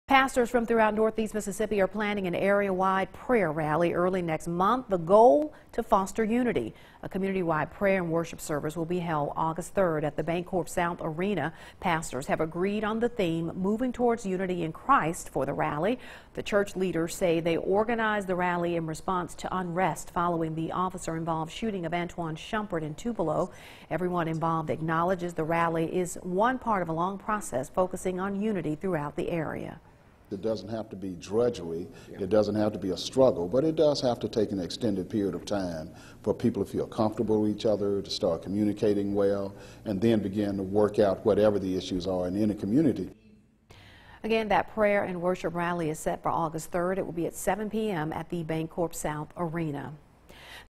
Pastor Meeting
Northeast Mississippi are planning an are-wide prayer rally early